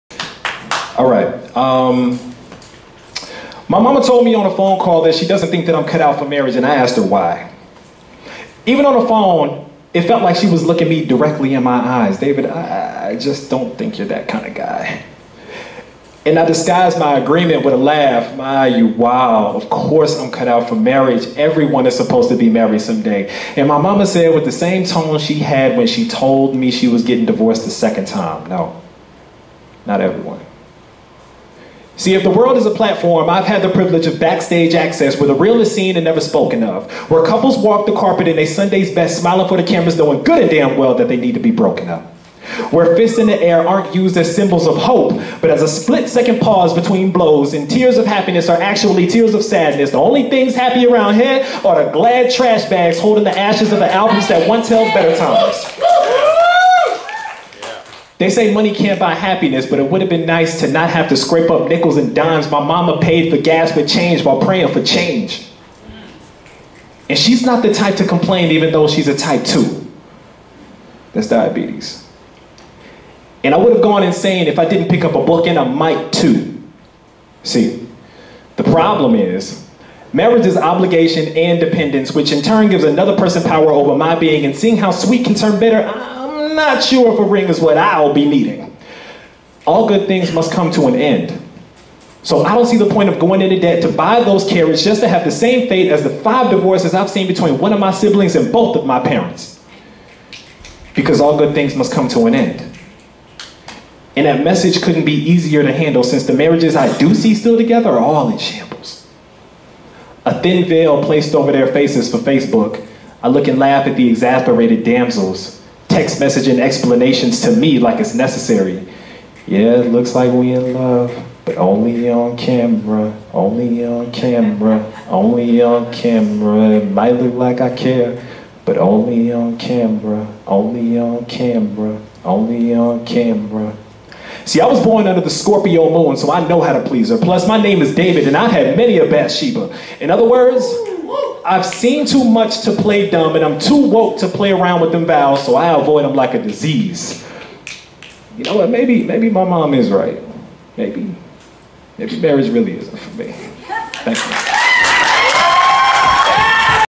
David Daniels - Marriage
David Daniels performing at the Out Loud HSV poetry slam on 8/13/17 in the Flying Monkey Theater in Huntsville AL